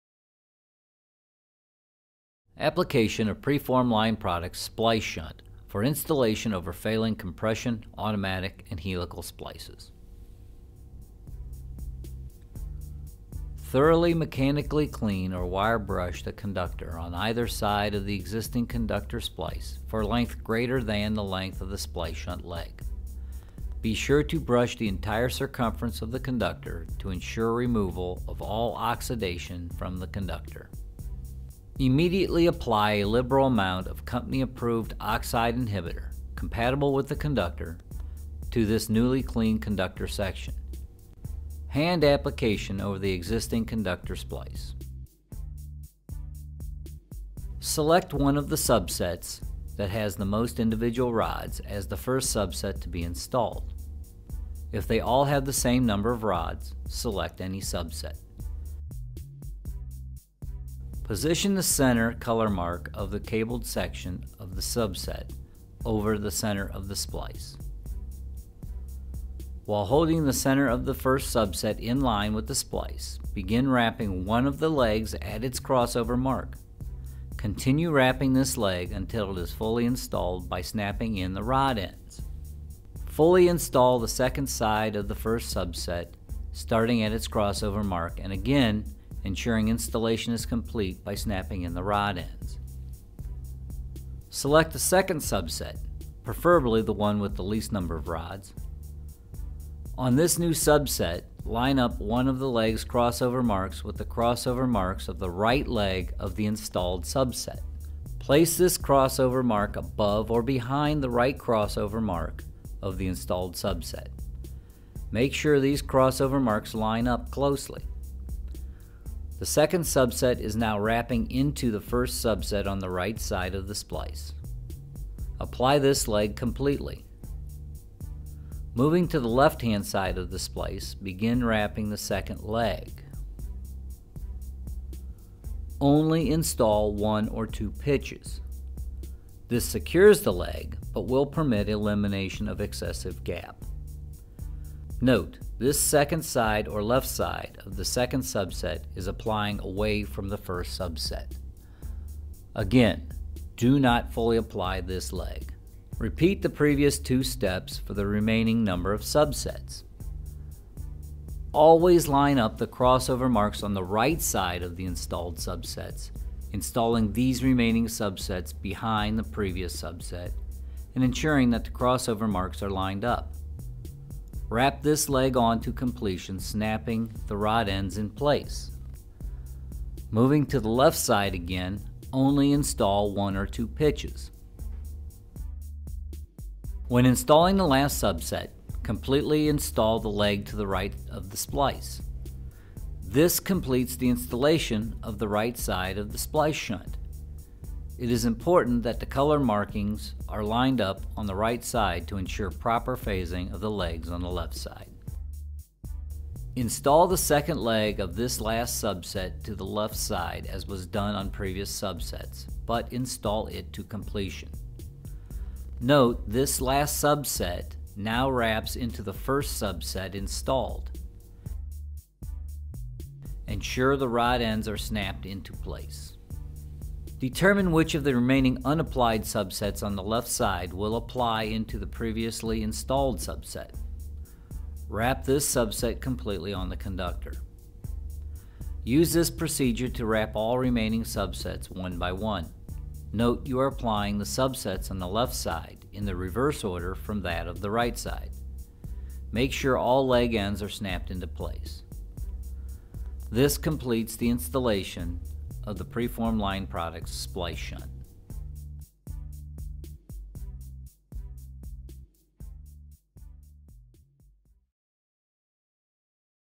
Splice Shunt Application for Installation Over Automatic and Compression Conductor Splices
The Splice Shunt is designed to restore complete electrical conductivity to automatic and compression splices that are found to be overheating and failing during thermal or resistive line inspections.